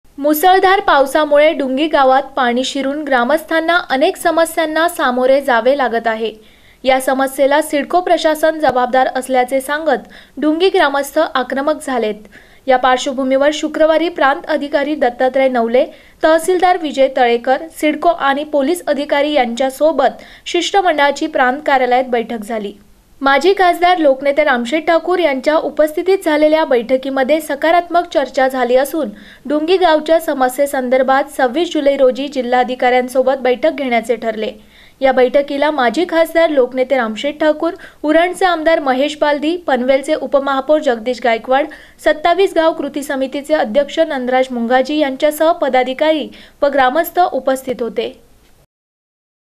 डुंगी ग्रामस्थांच्या समस्ये संदर्भात प्रांत कार्यालयात बैठक संपन्न !
डुंगी ग्रामस्थांच्या समस्ये संदर्भात माजी खासदार लोकनेते रामशेठ ठाकूर यांच्या उपस्थितीत प्रांत कार्यालयात बैठक संपन्न